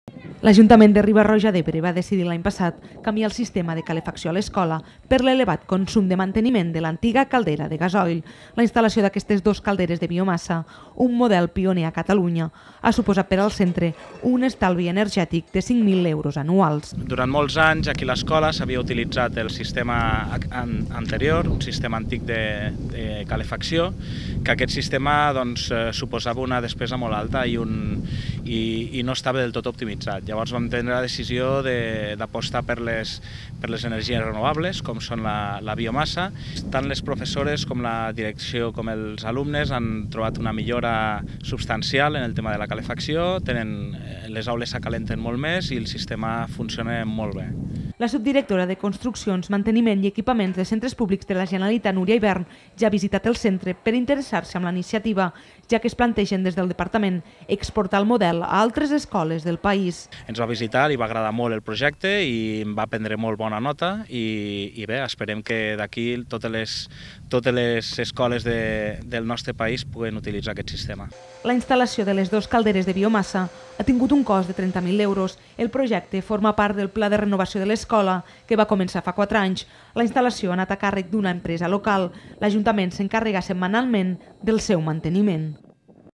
L’escola de Sant Agustí de Riba-roja d’Ebre compta amb un nou sistema de calefacció pioner en...
L’escola de Sant Agustí de Riba-roja d’Ebre compta des del curs passat amb un nou sistema de calefacció per biomassa, un model d’estalvi energètic pioner a Catalunya. La instal.lació de dos calderes de biomassa ha suposat que el centre reduís a la meitat la despesa energètica de l’edifici, uns 5000 euros anuals menys. Des del departament d’Ensenayament ja s’han interessat amb la iniciativa que estudien exportar a altres centres educatius.
L’ajuntament de Riba-roja d’Ebre va decidir l’any passat canviar el sistema de calefacció a l’escola per l’elevat consum de manteniment de l’antiga caldera de gasoil. La instal.lació d’aquestes  dos calderes de biomassa, un model pioner a Catalunya , ha suposat per al centre un estalvi energètic de 5000 euros anuals.
La subdirectora de Construccions, Manteniment i Equipaments de Centre Públics de la Generalitat, Núria Ivern, va visitar a finals d’any el centre per interessar-se amb la iniciativa ja que es plantegen exportar el model a altres escoles del país.  La instal.lació ha anat a càrrec d’una empresa local. L’ajuntament s’encarrega  El manteniment va
L’instal.lació de les calderes ha tingut un cost de 30 mil euros. El projecte forma part del pla de renovació de l’escola que va començar fa 4 anys. La instal.lació ha anat a càrrec d’una empresa local. L’ajuntament s’encarrega setmanalment del seu manteniment